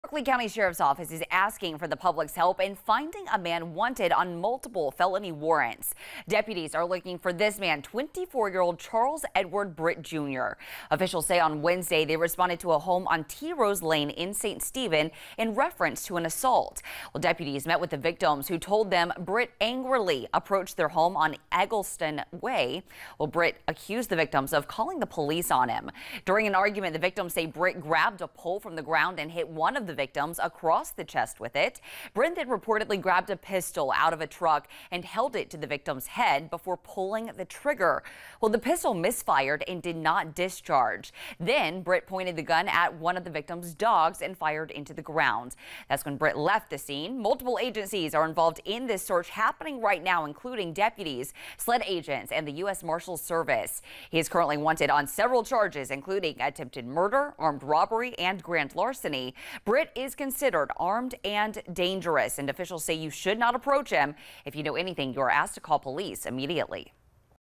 VIDEO: Berkeley Co. deputies searching for armed and dangerous wanted man
The Berkeley County Sheriff’s Office is asking for the public’s assistance in locating a man wanted on multiple felony warrants.